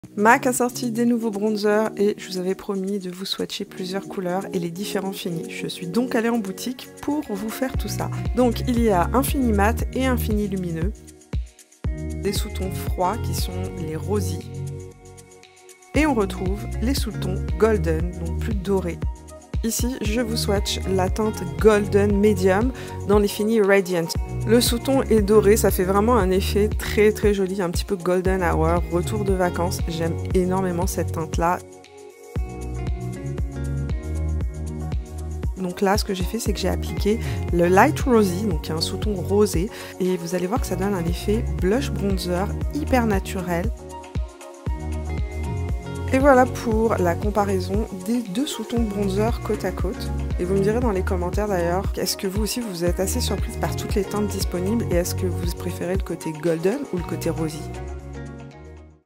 Skinfinish Sunstruck Radiant Bronzer Mac Cosmetics Swatch comparison
Swatch comparatif des nouveaux Bronzer Skinfinish Sunstruck Radiant de Mac Cosmetics

Produit de cette vidéo/ beauty Product in this video:
SkinfinishSunstruck, MAC Cosmetics, Radiant light Rosy ( mon préféré)